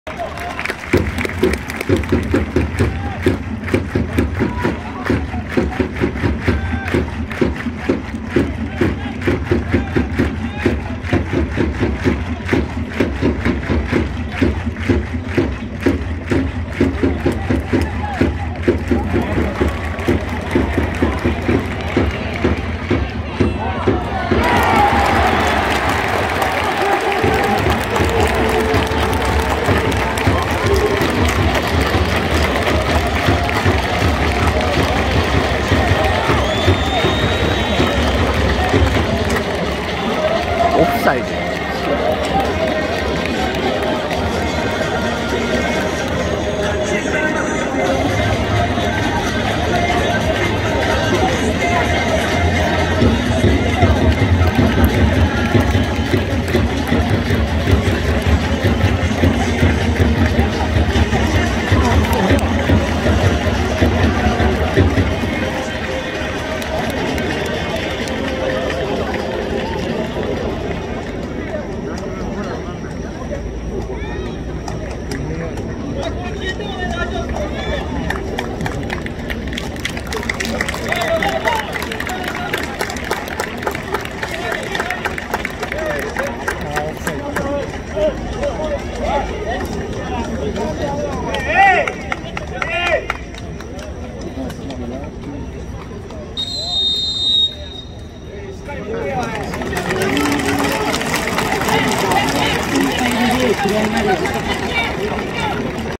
20210825 横浜FCvsガンバ大阪 あっという間の先制点前半1分 横浜せんせいごーーーーーーーる は無しよ笑(VAR)
20210825 横浜FCvsガンバ大阪　あっという間の先制点前半1分　横浜せんせいごーーーーーーーる　　　　は無しよ笑(VAR)